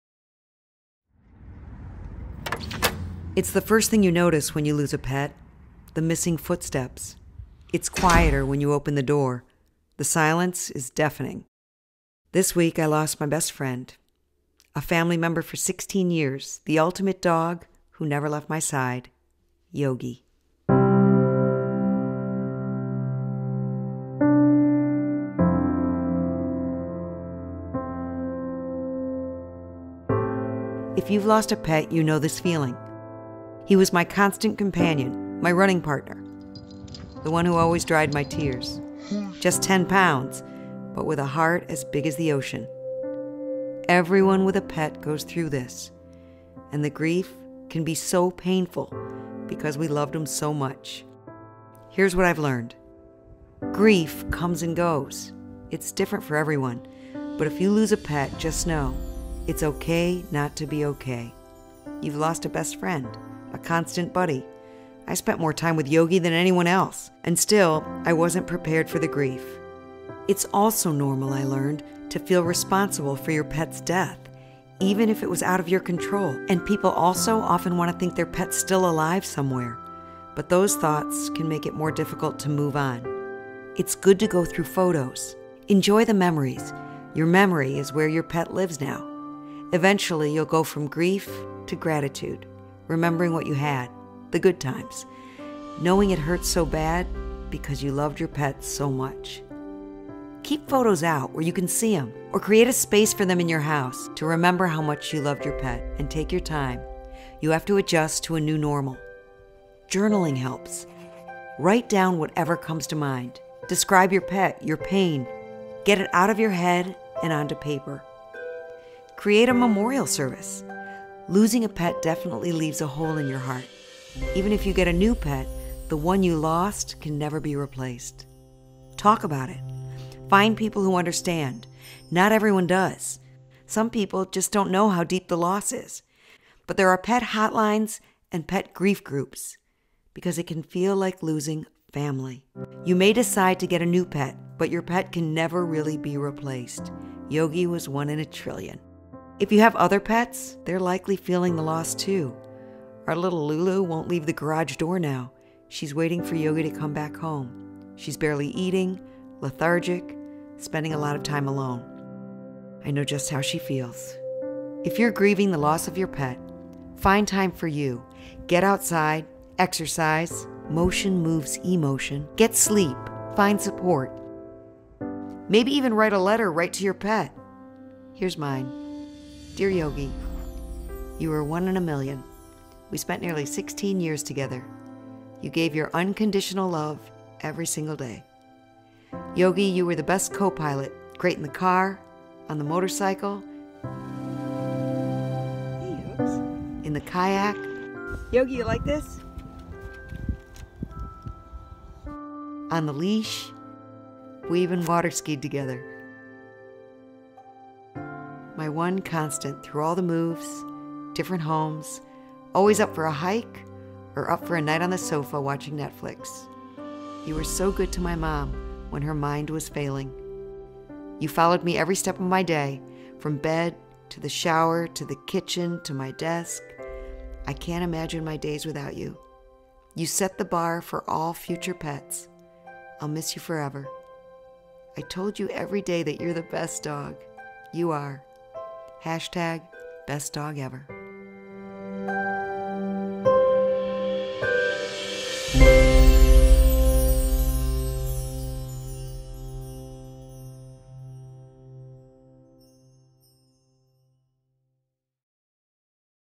Losing Yogi | with Lila Lazarus
How to Survive the Loss of A Pet

It’s heart-wrenching.  The more you love your pet, the more painful it can feel when they’re gone.  Losing a pet can be very difficult.  They’re more than just animals.  Often, they are beloved members of the family.  In this video, Gracefully Greying health correspondent Lila Lazarus tells the story of losing her dog, Yogi, and shares tools for getting through the grief.